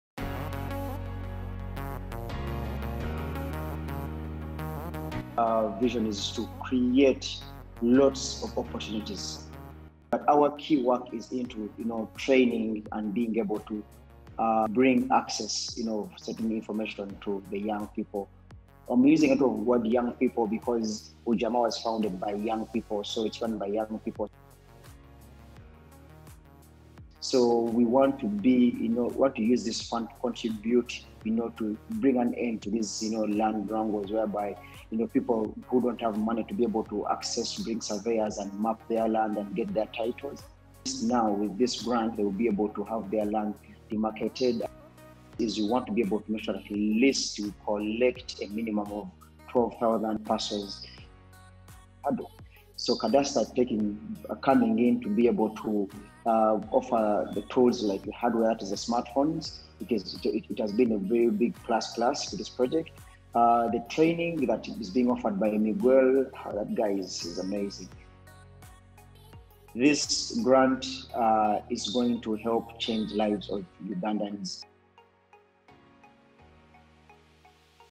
Talking with Cadasta Grantees: Ujamaa Tribe
Ujamaa Tribe is the third organization this fall to receive a grant under Cadasta’s Global Land Rights Challenge Fund. The organization will use the funds, along with technology and training from Cadasta, to map and collect data from 1,200 households in the Namutumba District of Uganda.